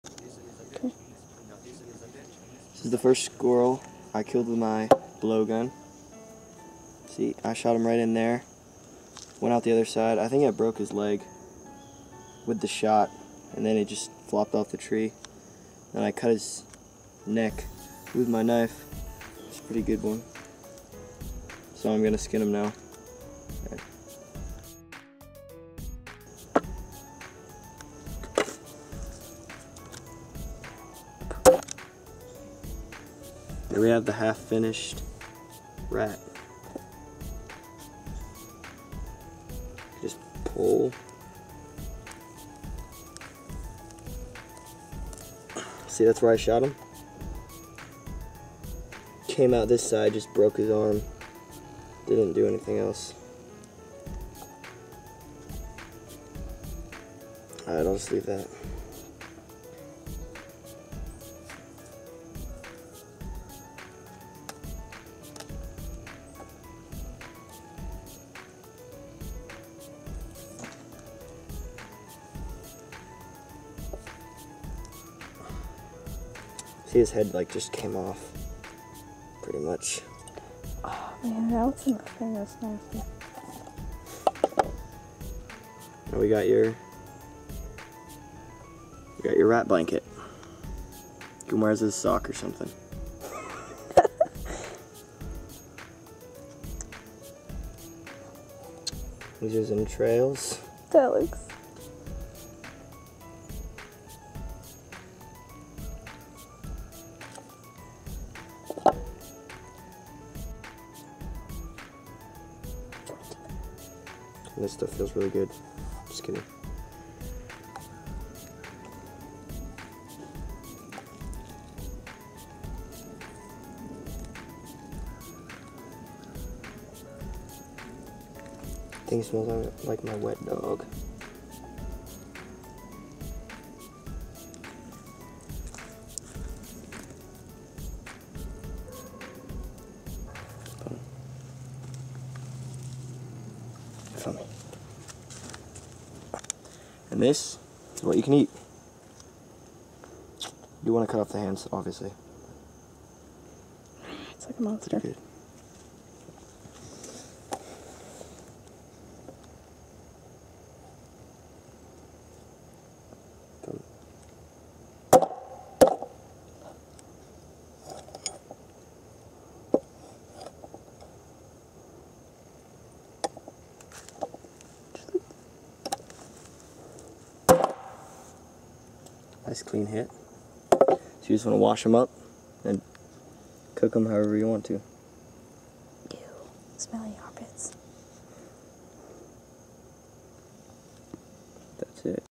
First Squirrel Kill With Blowgun - Cleaning Squirrel
I shot him but didn't have the camera on me. I Clean him in this video. This isn't the best tutorial on how to clean a squirrel but we'll make a better one.